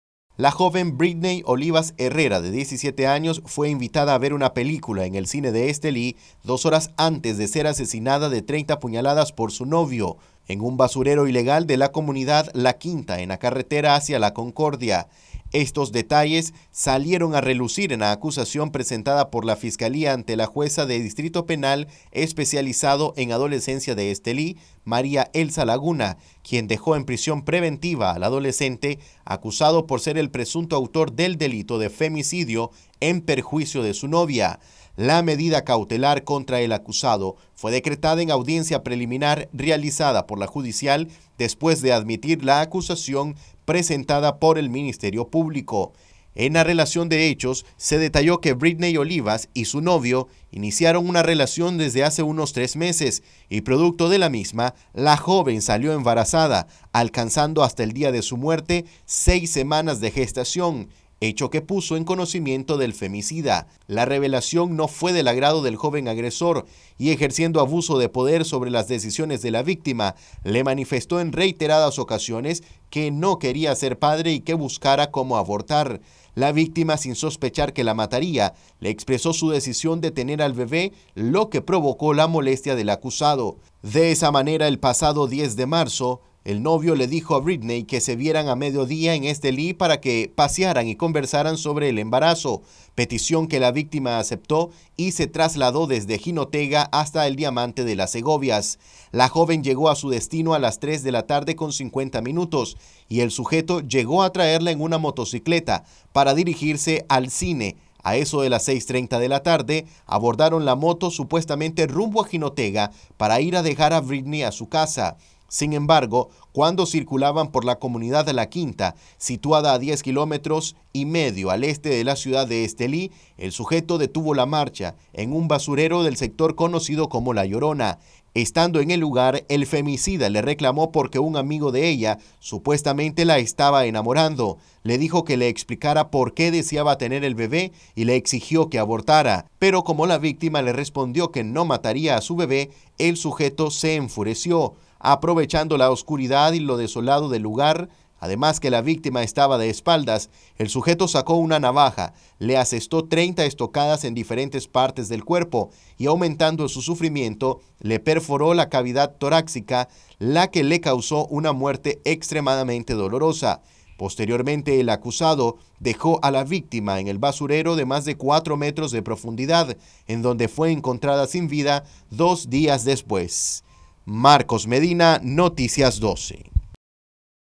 Acusación contra femicida revela nuevos datos sobre el crimen contra Britney Olivas
Noticias12Nicaragua Acusación contra femicida revela nuevos datos sobre el crimen contra Britney Olivas